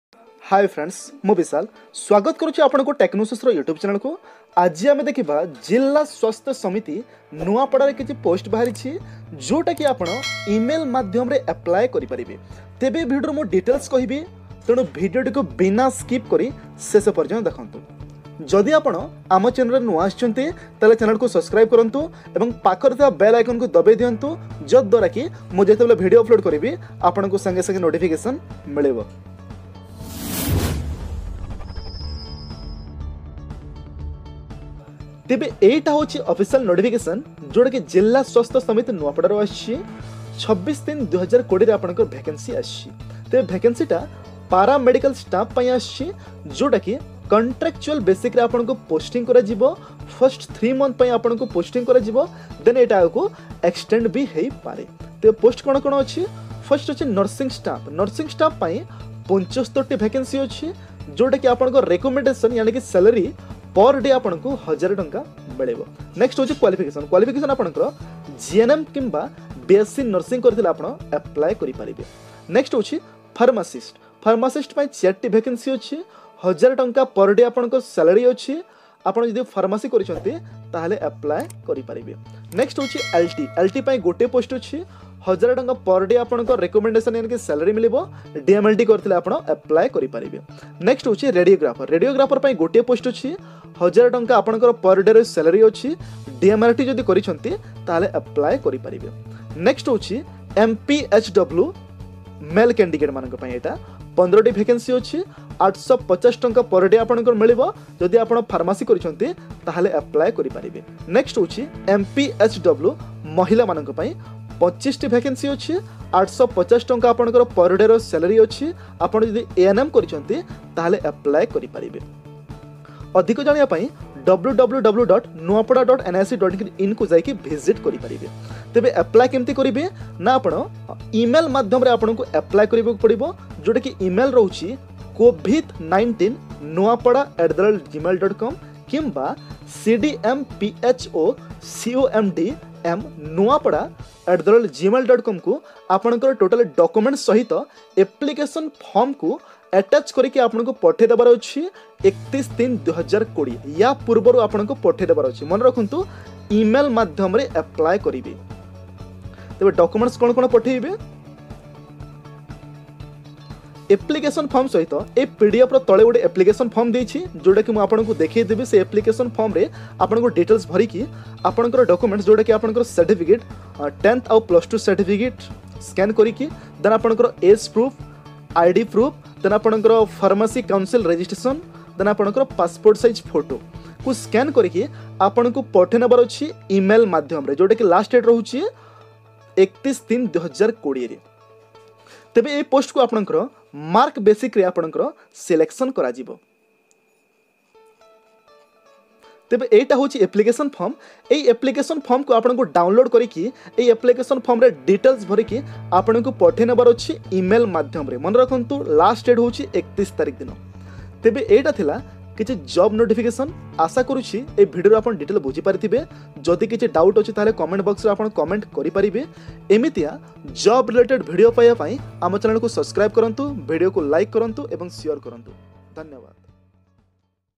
ଜିଲ୍ଲା ସ୍ୱାସ୍ଥ୍ୟ ସମିତି ନୂଆପଡା ରେ ନିଯୁକ୍ତି ସୁଯୋଗ 2020 ,, Covid19 Recruitment Nuapada
Jilla swasthaya samiti nuapada Recruitment 2020
post - Staff Nurse, Pharmacist, LT, Radiographer, MPHW